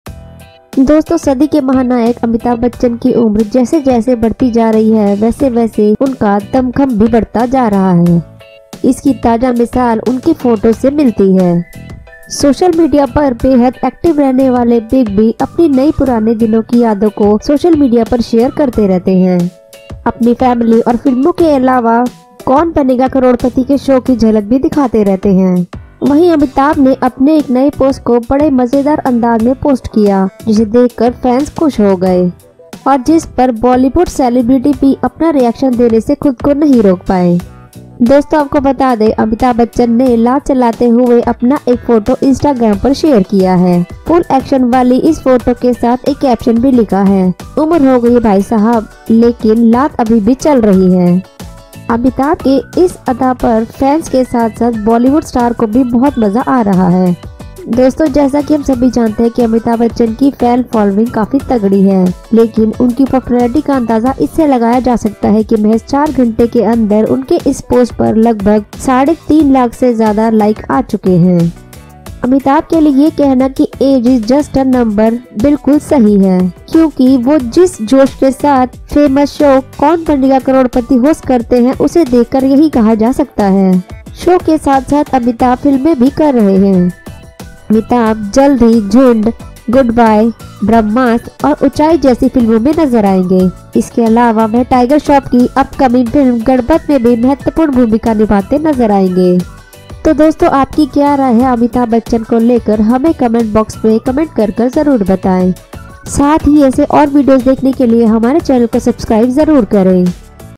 अमिताभ बच्चन ने अपनी फोटो शेयर कर कहा उम्र हो गई है भाई साहब
B24 Bollywood for Hindi Entertainment News, Bollywood gossips, Tv facts, Tv affairs, Short videos and much more.